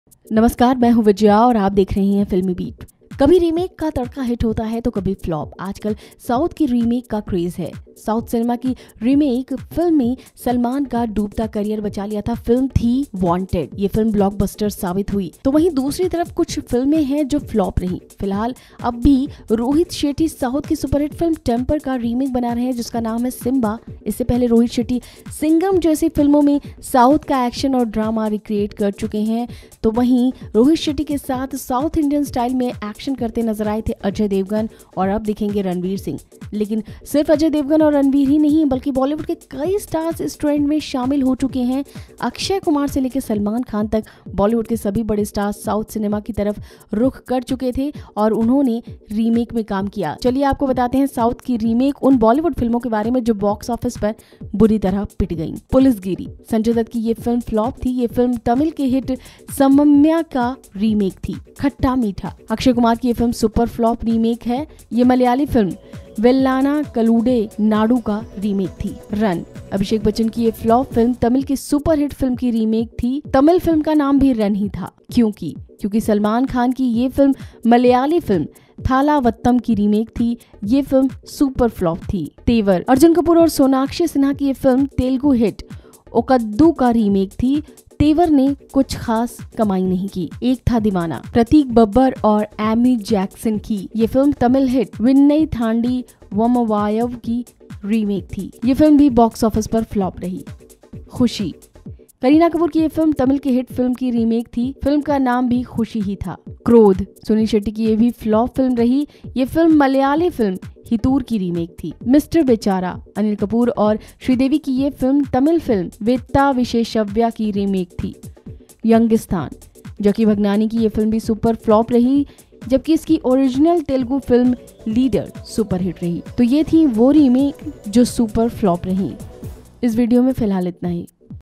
Akshay Kumar, Sanjay Dutt & others deliver WORST Bollywood Remakes of South Indian Movies |FilmiBeat
There is a tradition of Hindi films borrowing content from the South Indian movies. Some directors remake their own Southern classics into Hindi features keeping in mind the business and exposure aspect. Rohit Shetty's Simba, which is inspired by the Telugu hit, Temper. But here in this video know about the flop Bollywood movies which were inspired by south or remake.